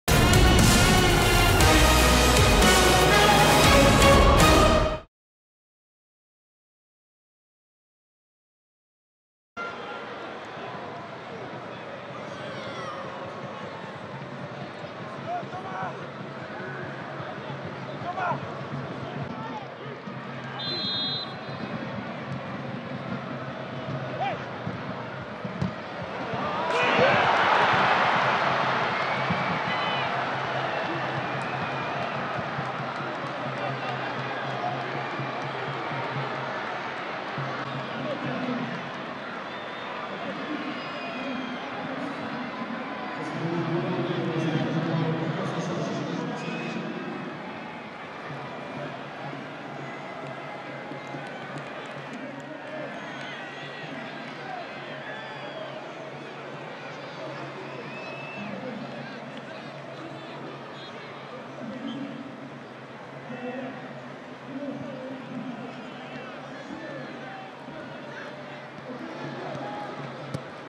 But Georges MANDJECK (48') / Toulouse FC - FC Metz (1-2) - / 2016-17
Toulouse FC - FC Metz (1-2) - But de Georges MANDJECK (48'). Tous les buts de Toulouse FC - FC Metz en vidéo.

Stadium - samedi 19 novembre 2016

Buteurs : Yann JOUFFRE (35' pen - FC Metz) / Georges MANDJECK (48' - FC Metz) / Odsonne EDOUARD (90' +5 - Toulouse FC)